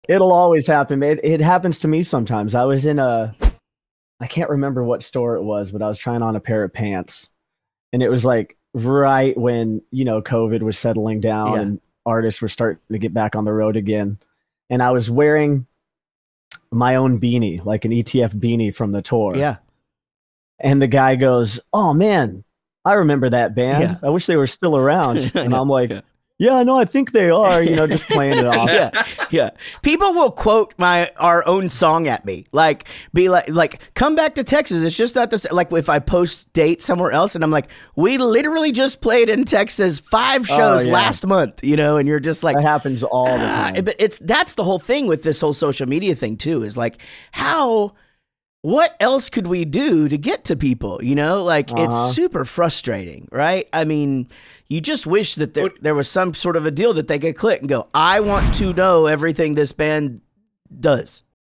Craig Mabbitt (Escape The Fate)
The Rockstar Dad Show - Est. 2018

On this week’s show, Jaret & Gary talk to Craig Mabbitt from the amazing band, Escape The Fate

TONIGHT at 9pm ET